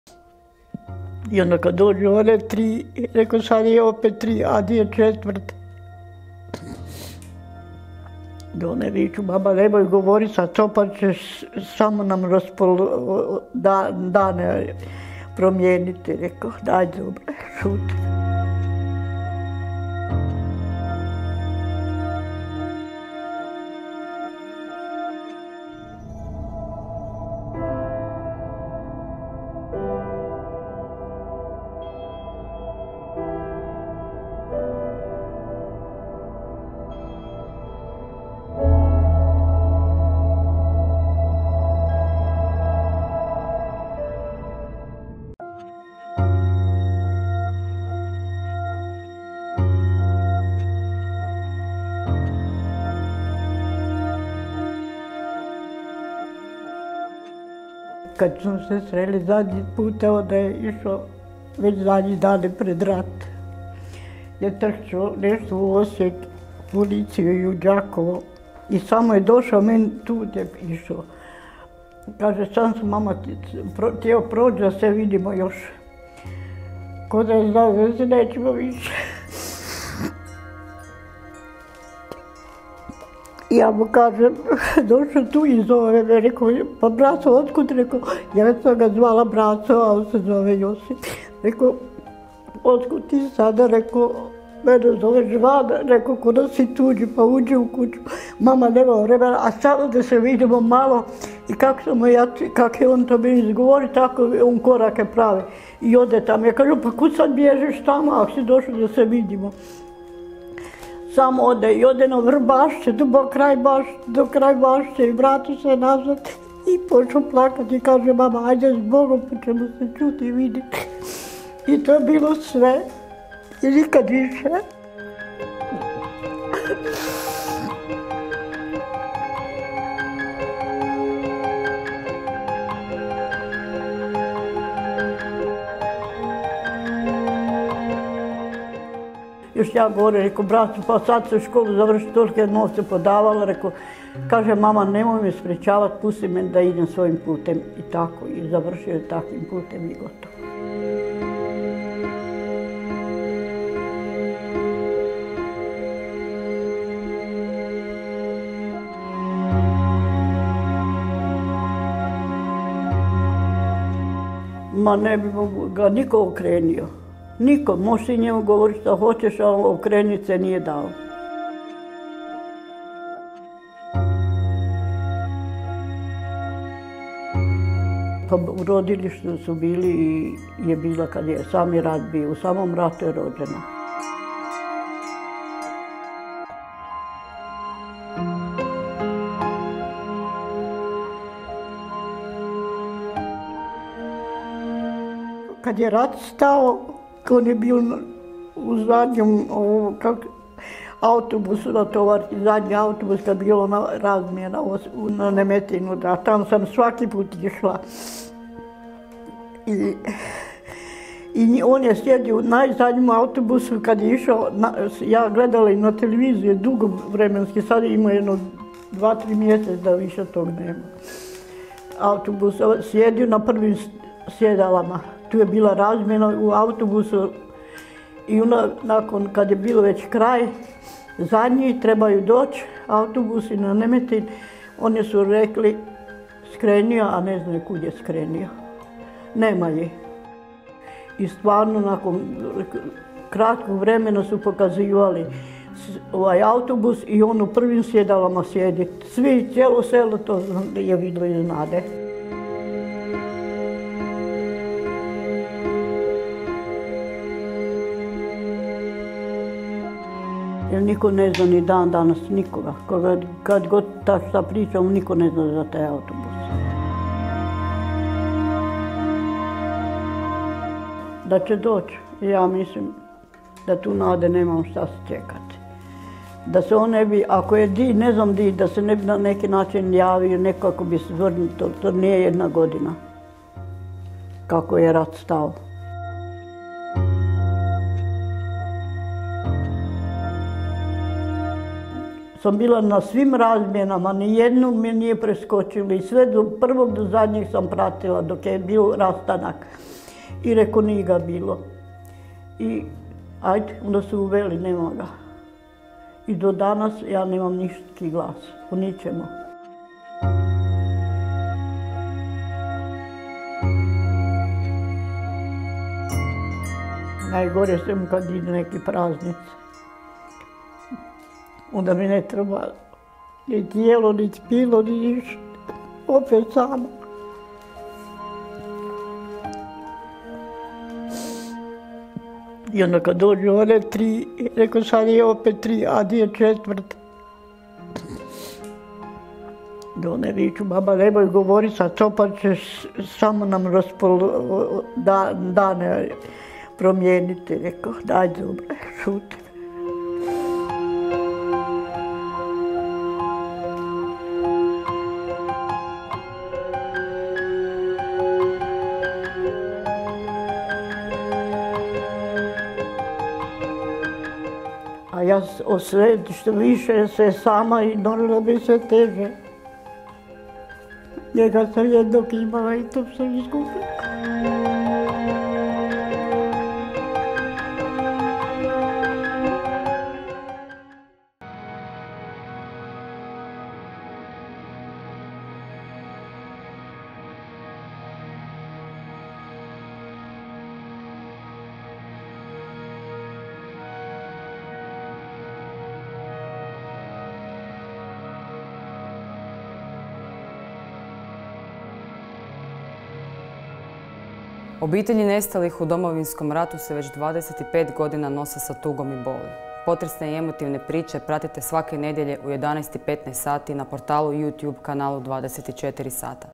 Milena tuguje za sinom Josipom: 'Odgovorite mi kamo ste odveli mojeg sina?'
Milena Rep saznala je da je Josip završio u Sremskoj Mitrovici. Vidjeli su ga u autobusu, no nestao je s rute. U Nemetin nikad nije stigao.

Imate priču?